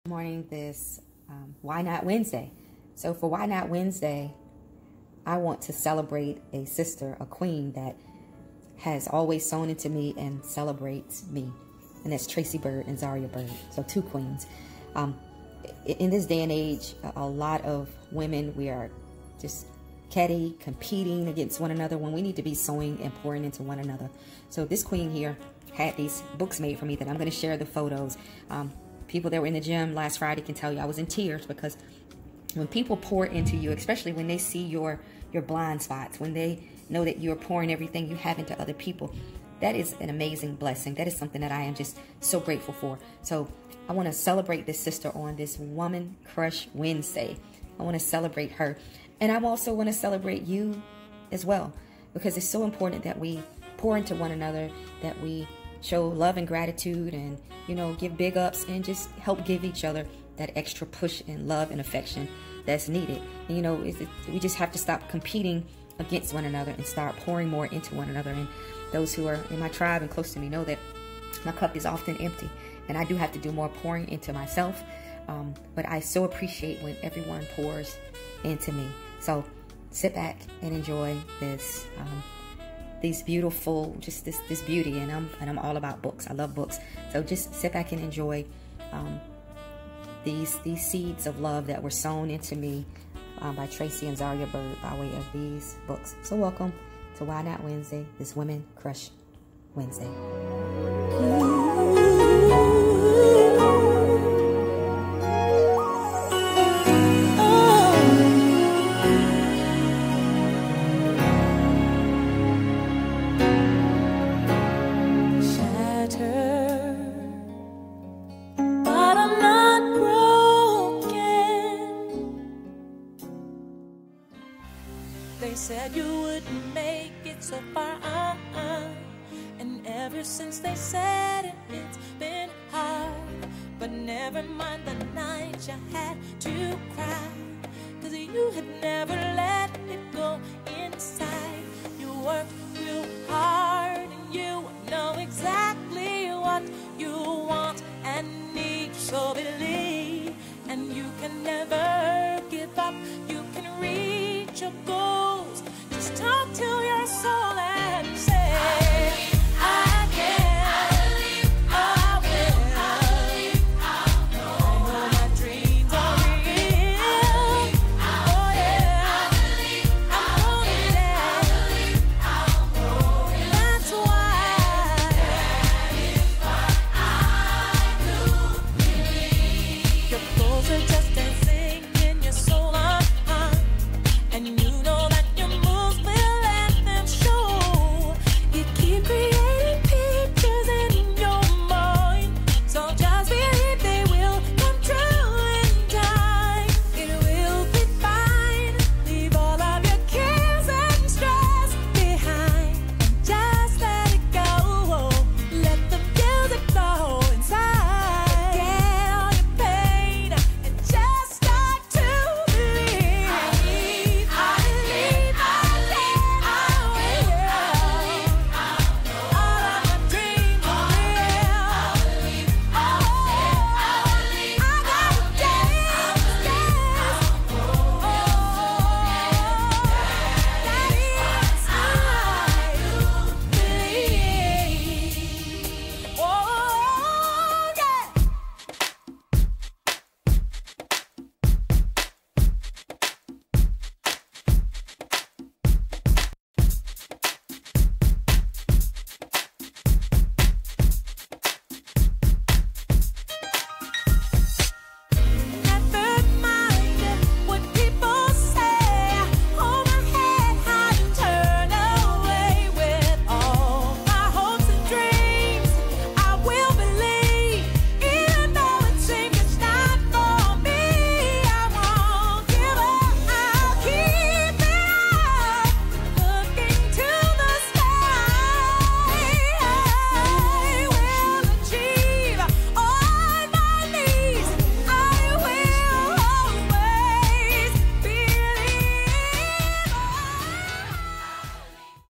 OCB pro Card 9/12/2020
On this WCW I celebrate Tracey and Zaria Byrd.

They Sowed seeds of Love ❤ into me by pouring into my cup with these beautiful books recapping my journey as
I manifested and put in the work to win an OCB Pro Card figuremodel!

I AM grateful, Thankful & Blessed and will forever cherish these books!  I Love and appreciate my entire Be Your Own Art Tribe!

I AM NOT Lucky; I AM Blessed!
I AM NOT Lucky; I AM Loved!